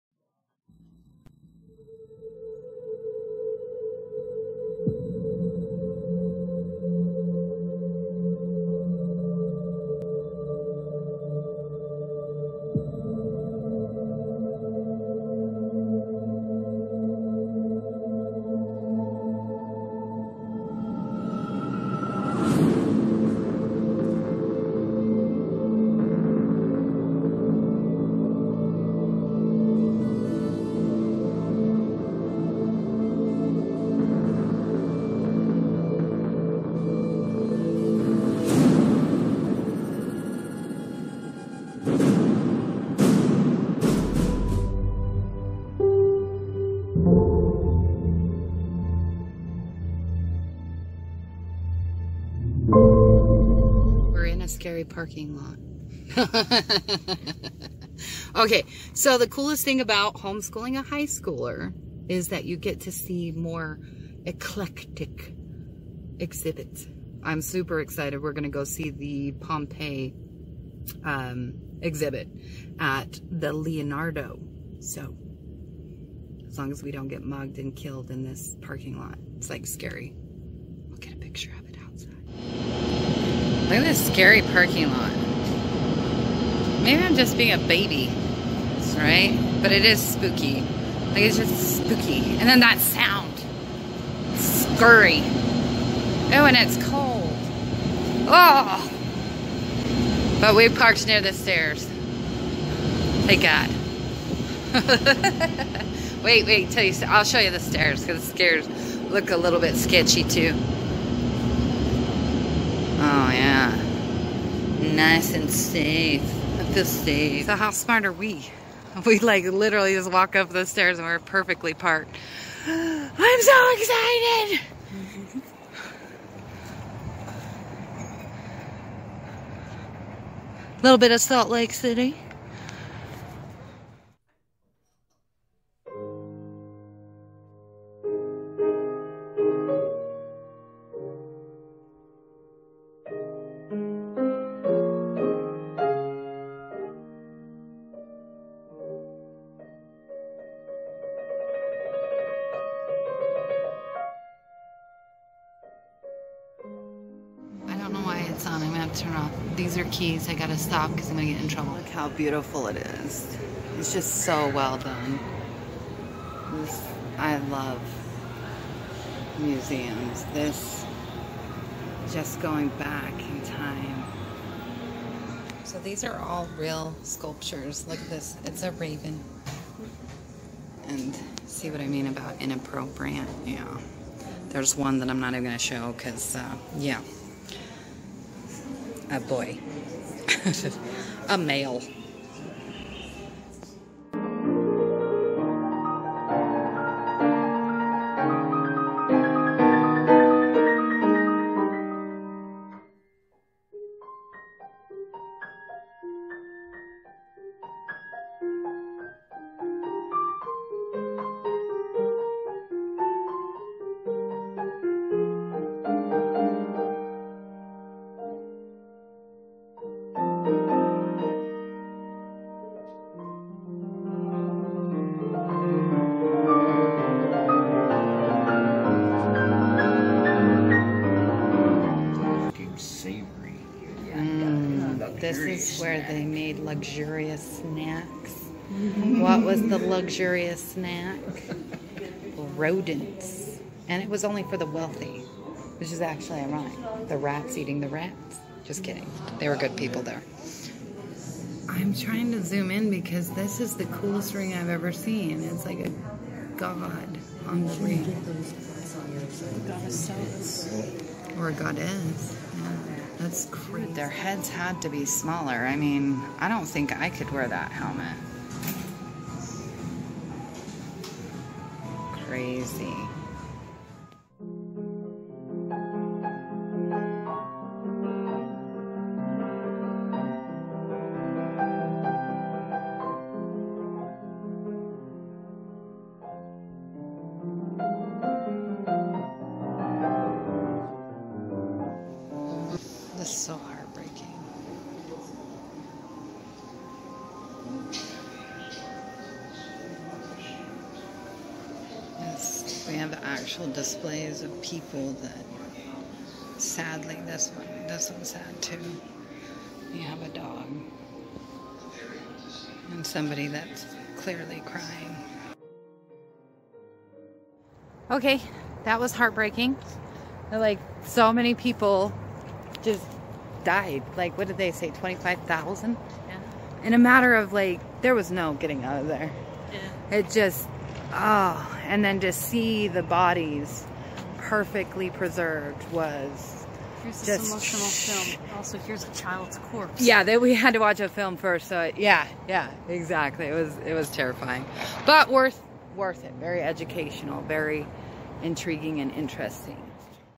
Pompeii: The Exhibition | The Leonardo | VLOG
Got to see the Pompeii Exhibit in Salt Lake City at the Leonardo.  So worth going, but would advise teens and above only.  The second half of the Tour is Heartbreaking.